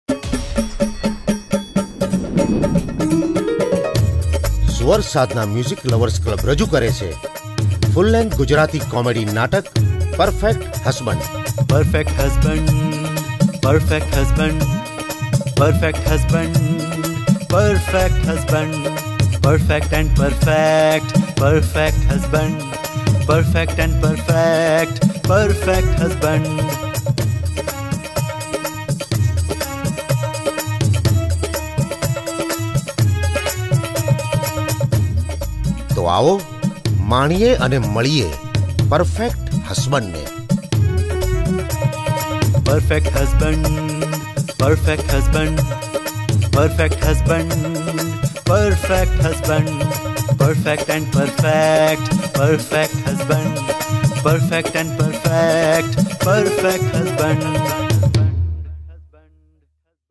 Perfect Husband - Gujarati Drama in Brampton on July 30, 2023
Secure your seats now for the full length Gujarati Drama, Perfect Husband!
📅 On Sunday, July 30th, 2023 @ 5:30 PM
📍 Lester B. Pearson Theatre, Brampton
🎟 Tickets available now at Sulekha.com

Written by Kaajal Oza Vaidya & Directed by Akhil Pandya